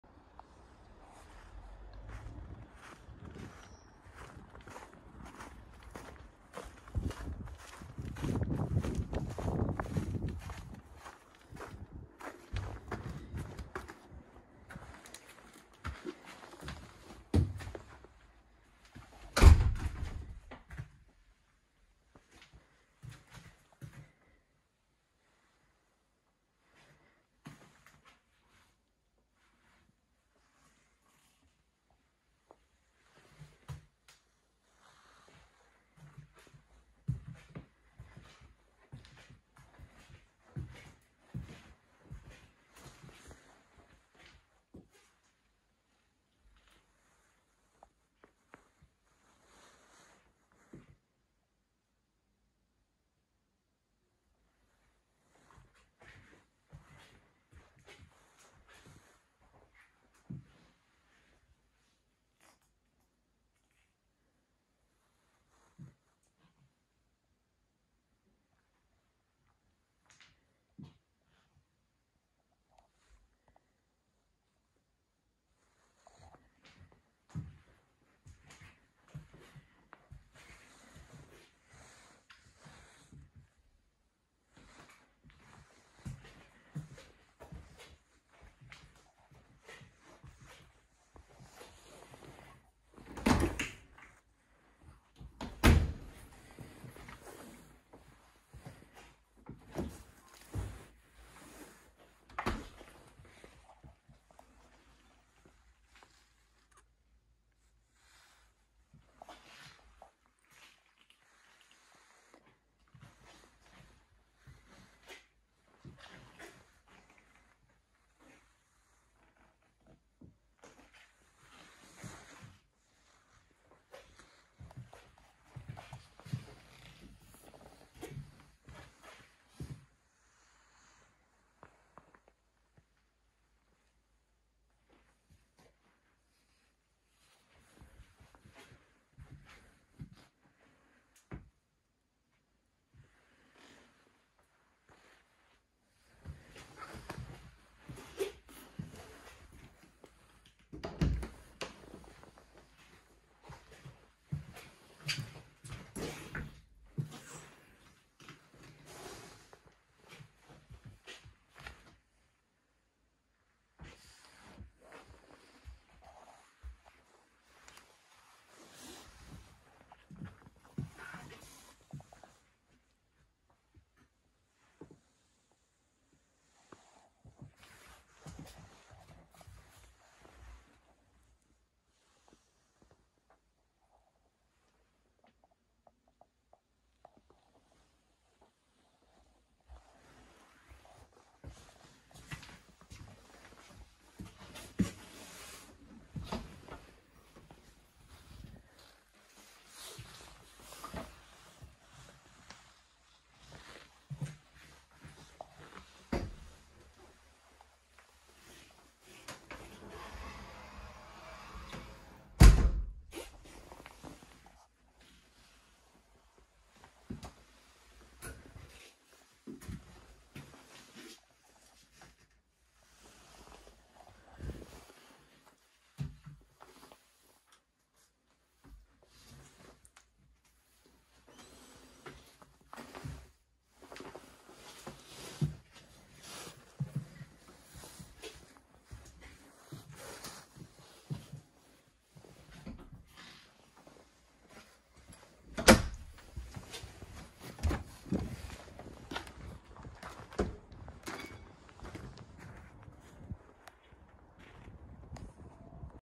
Unser winterfestes Microhaus Modell Rügen.
Auf 45 qm kann man sich auch wohlfühlen. Dieses Haus ist als ein kleines Ferienhaus geplant. Irgendwo am Wasser oder in den Bergen. Es gibt aber auch Menschen, die sich verkleinern möchten und gezielt in einem 45 qm Microhaus leben. Das Chalet kann man bei uns auf dem Schaugelände in Flensburg besichtigen.